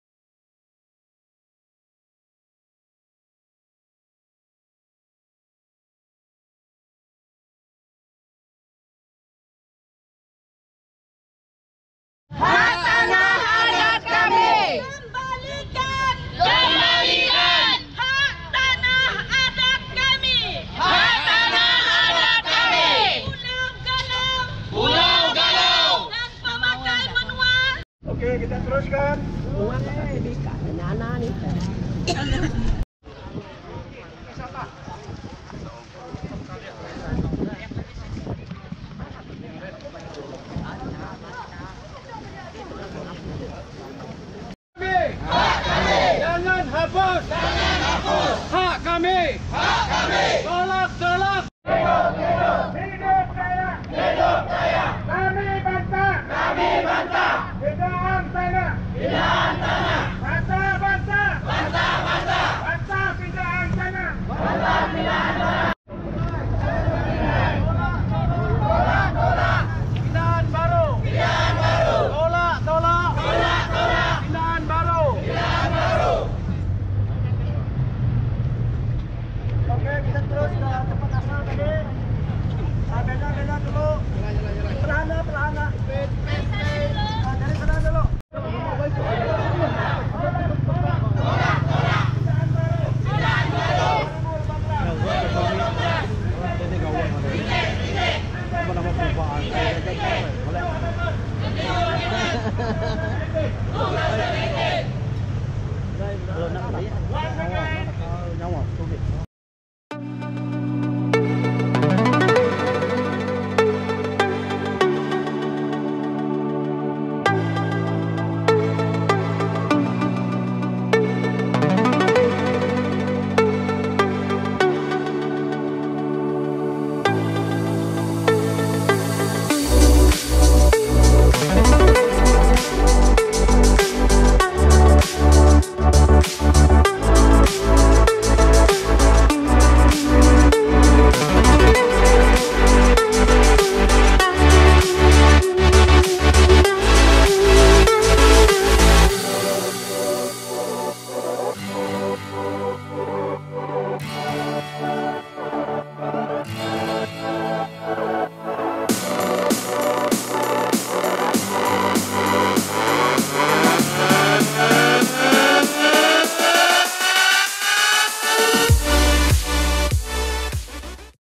NO to Landcode Amendment 2018 | 7.7.2018
we oppose the new Landcode Amedment 2018

SAY NO to Landcode Amendment 2018!!!